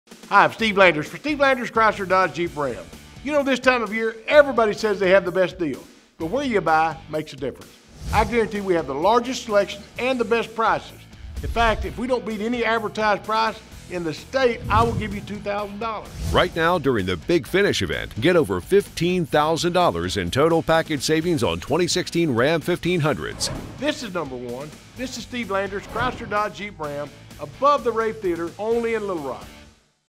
Where You Buy | Steve Landers Chrysler Dodge Jeep Ram
Steve Landers Chrysler Dodge Jeep Ram
401 Colonel Glenn Plaza Loop
Little Rock, AR 72210

This time of year, everybody says they have the best deal, but where you buy makes a difference. If we don't beat any advertised price in the state, we'll give you $2,000.